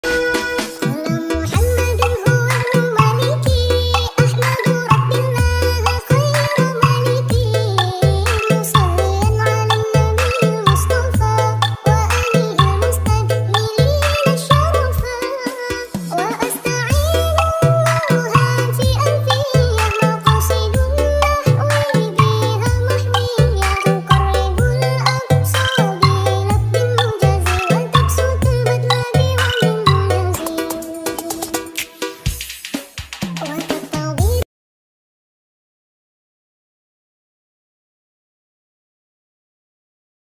KUCING PINTAR BACA SHOLAWATAN
ANEH TAPI NYATA !! KUCING PINTAR BISA BACA SOLAWATAN

video ini hanya hiburan dan editan dari kecanggihan sistem  AI yang semakin maju, tonton video nya sampai selesai ya sobat. dan jika kalian suka , dukung terus channel ini ( sagalaaya14 ) dengan cara :

HATURNUHUN 🙏